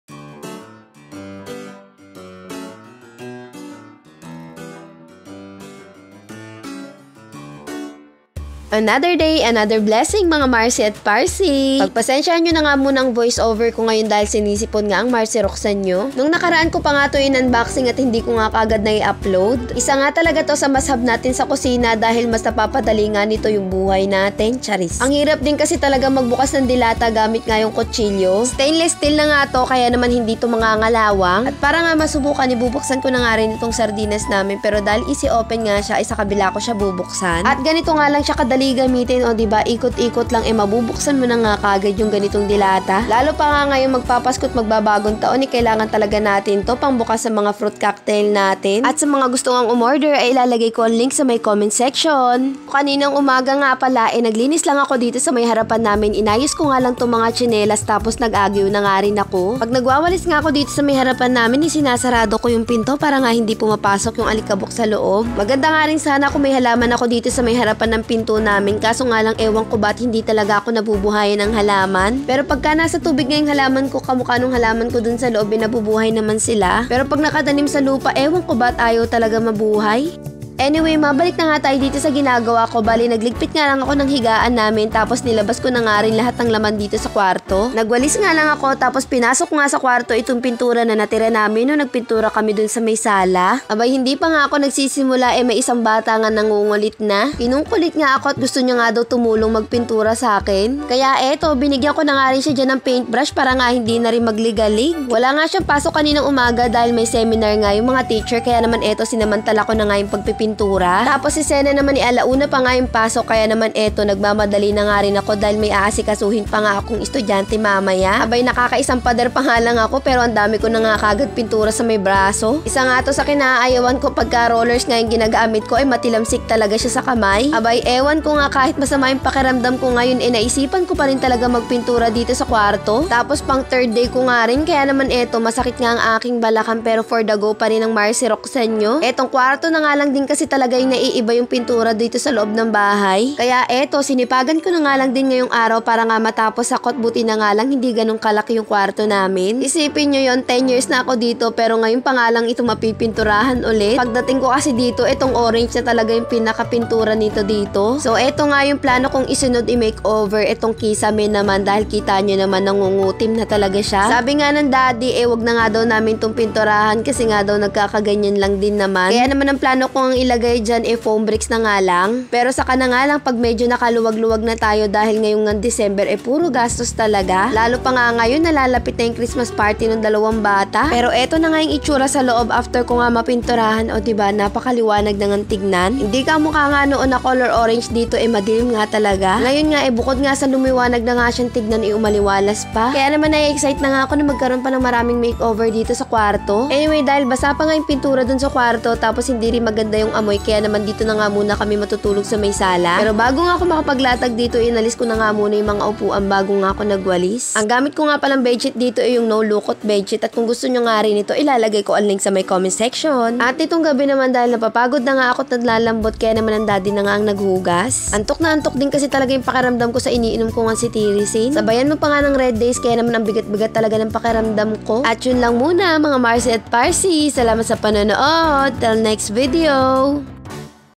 Isunod naman nating imake over ang kwarto, samahan nyo ko magpintura🥰
Isunod naman nating imake over ang kwarto, samahan nyo ko magpintura🥰 Can't wait to fully make over it🤍🪵